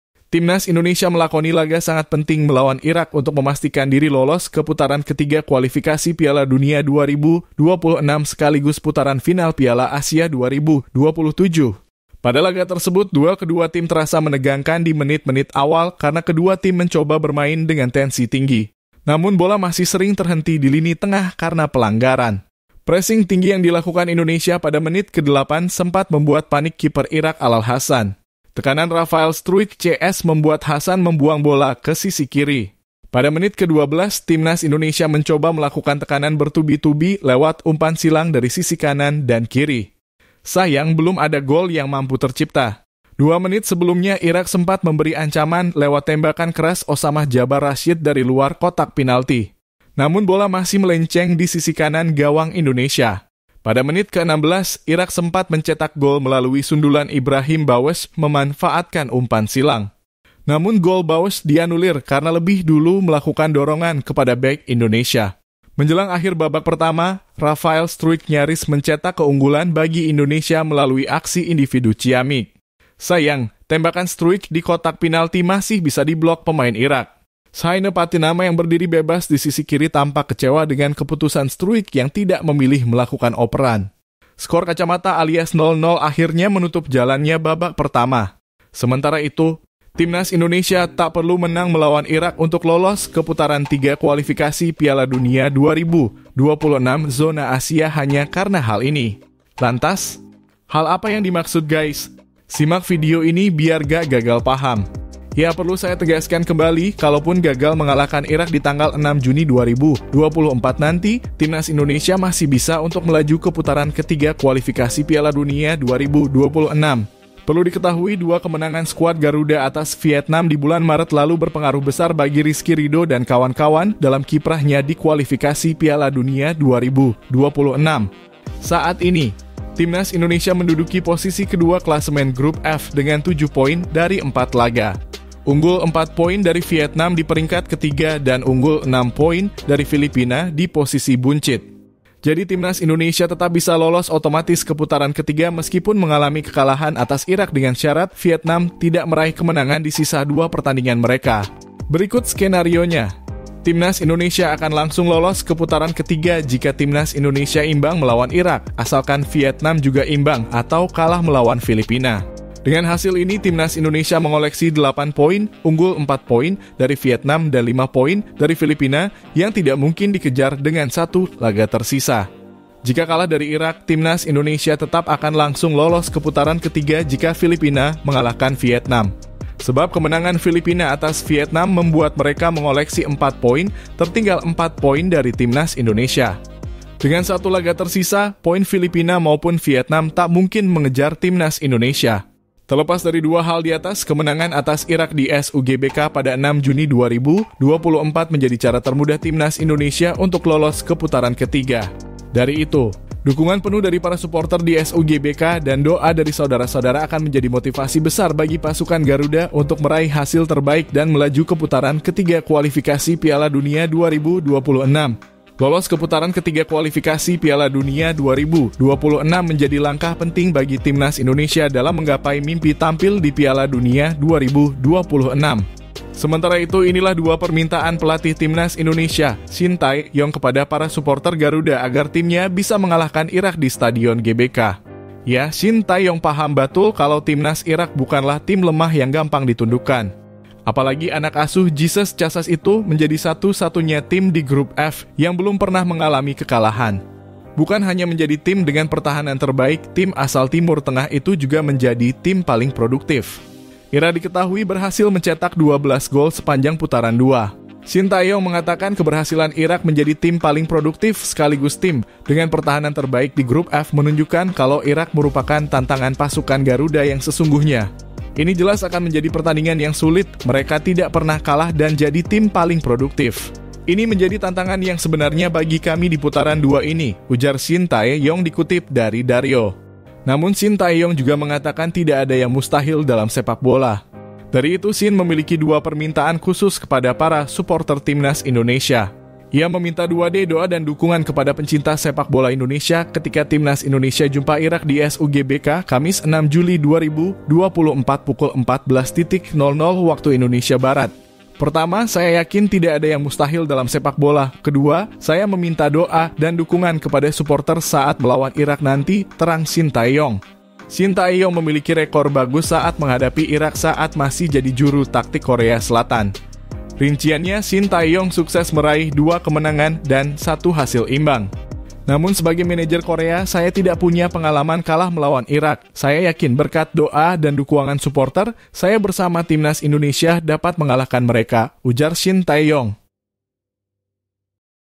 🔴YES TETAP LOLOS PUTARAN 3 ~ Hasil Indonesia Vs Irak 0-2 Di Kualifikasi Piala Dunia ~ Vietnam Nangis
Dapatkan Koleksi Jersey Terbaik Timnas Indonesia Terbaru Dengan Cara Klik Link di Bawah Ini